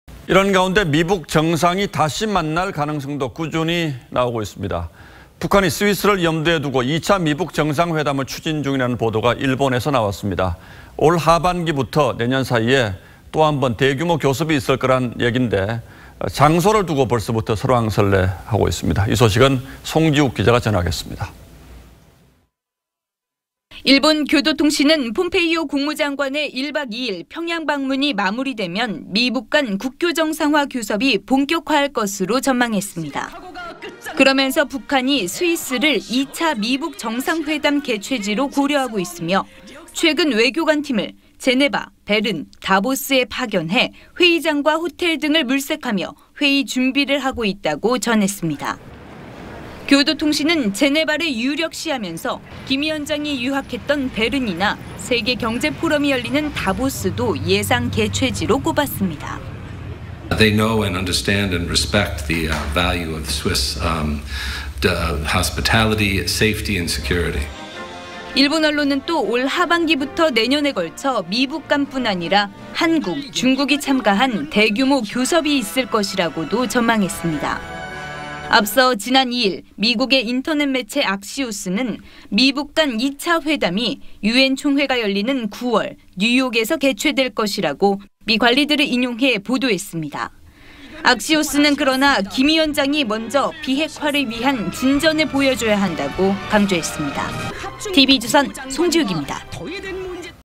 일본 언론 "북한, 2차 미북 정상 회담 장소로 스위스 검토"
이런 가운데, 미북 정상이 다시 만날 가능성도 꾸준히 제기되고 있습니다. 북한이 스위스를 염두에 두고 2차 미북 정상 회담을 추진 중이란 보도가 일본에서 나왔습니다. 올 하반기부터 내년 사이, 또 한 번의 대규모 교섭이 있을 거란 건데, 장소를 두고 벌써부터 설왕설래입니다.